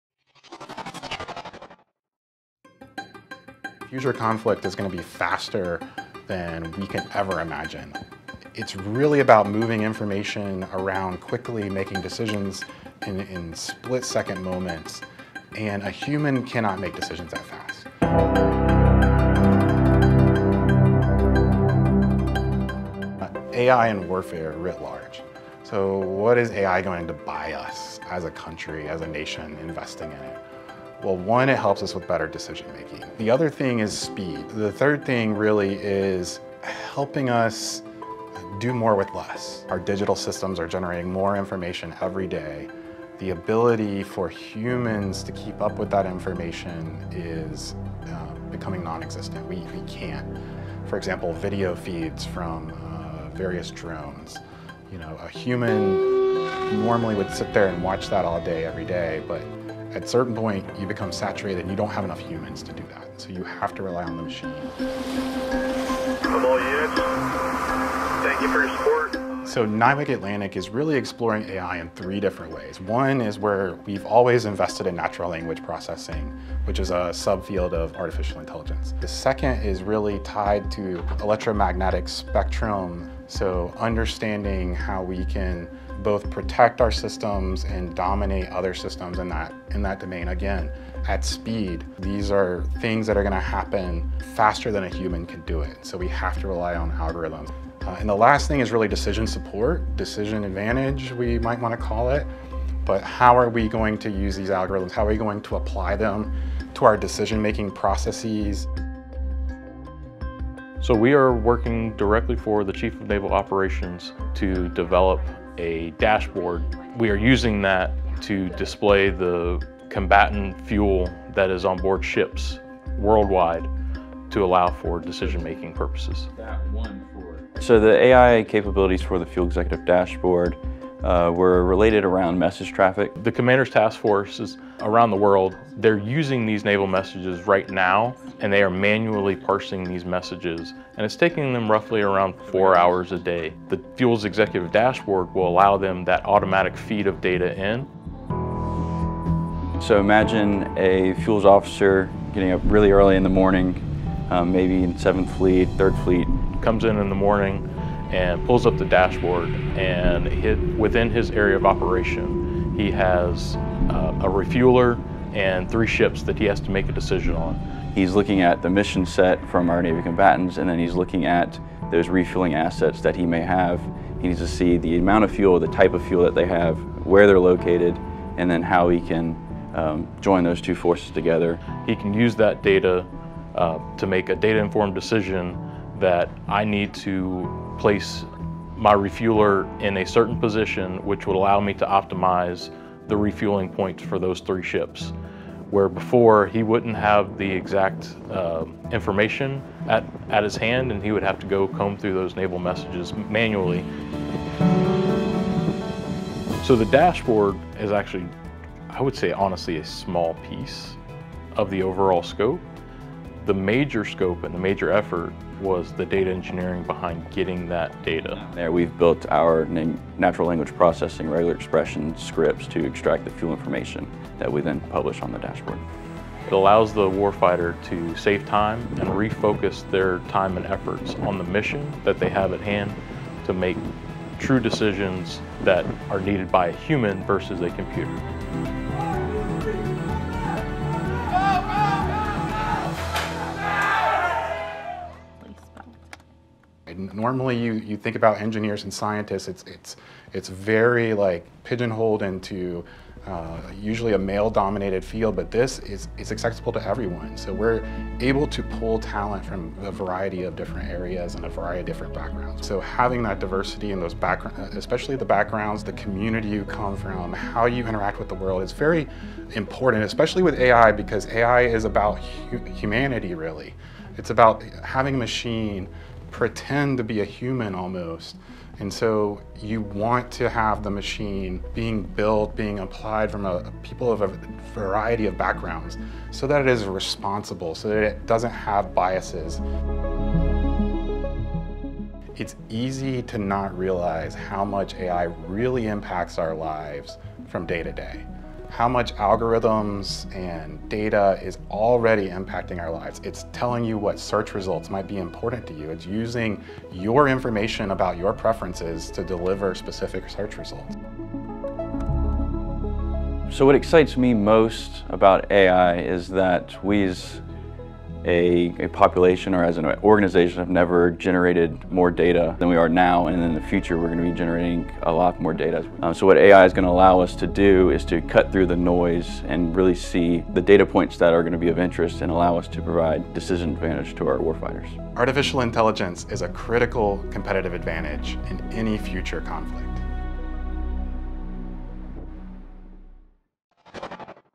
Artificial Intelligence and Warfare with NIWC Atlantic
This is a video feature that describes the benefits of the partnership between Artificial Intelligence and strategic capabilities within warfare. NIWC Atlantic's Artificial Intelligence programs range in capabilities within machine learning to language processing and more. The programs, tools, and developments created at NIWC Atlantic assist in strategic decision-making in many areas of the Department of Defense; including but not limited to, warfare strategy. Currently, NIWC Atlantic is working on a Fuels Executive Dashboard that uses AI to assist fuels data collection across the Navy.